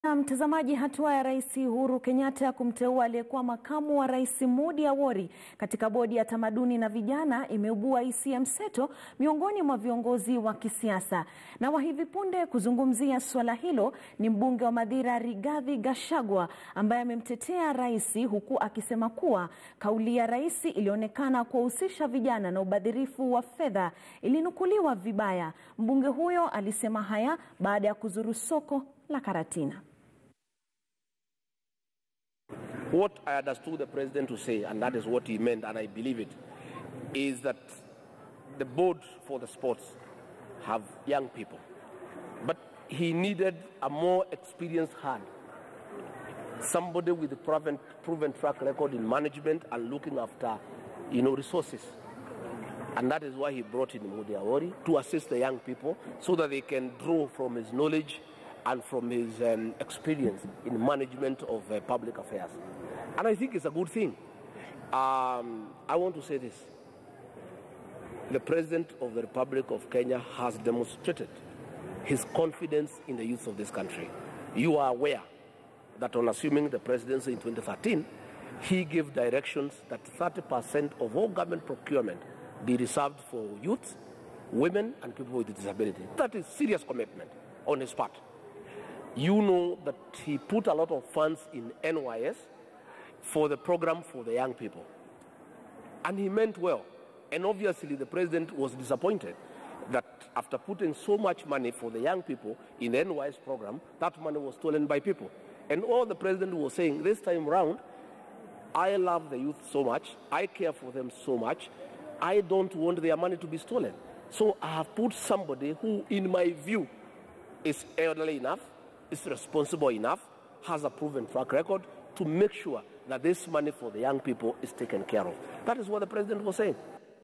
Mbunge Rigathi Gachagua amemtetea Rais Uhuru Kenyatta kwa kumteua Moody Awori
Hatua ya Rais Uhuru Kenyatta ya kumteua aliyekuwa Makamu wa Rais Moody Awori katika bodi ya tamaduni na vijana imeibua hisia mseto miongoni mwa viongozi wa kisiasa. Wa hivi punde kuzungumzia swala hilo ni Mbunge wa Mathira Rigathi Gachagua ambaye amemtetea  rais huku akisema kuwa kauli ya rais ilioonekana kuwahusisha vijana na ubadhirifu wa fedha ilinukuliwa vibaya. Mbunge huyo aliyasema haya baada ya kuzuru soko la karatina.